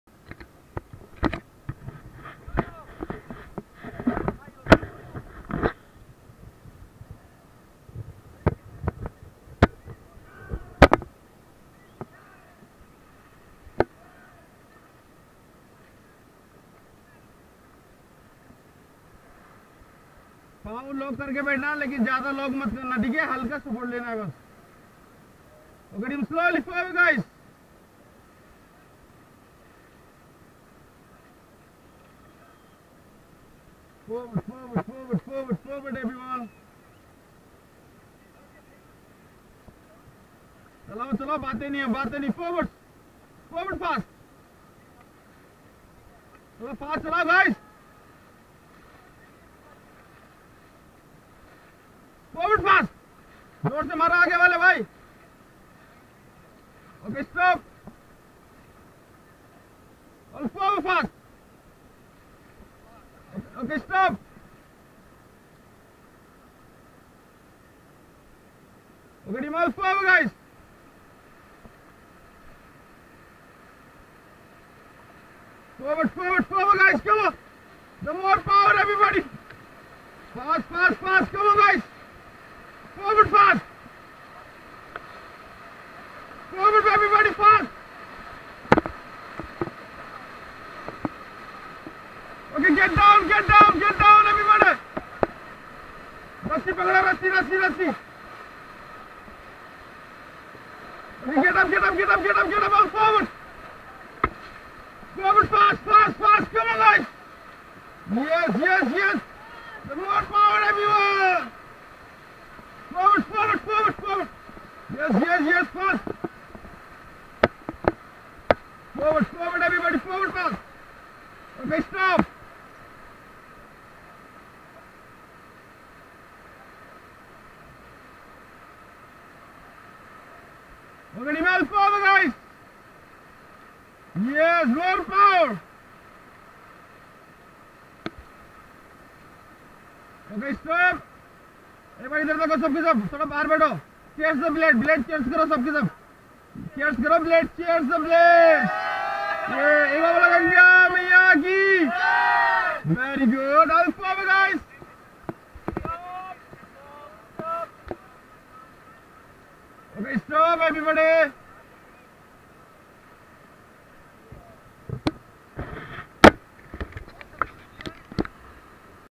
Rishikesh river rafting in fast current
River rafting in rishikesh, mission 25 km. Boat is Crossing extremely fast current of the river holy Ganga.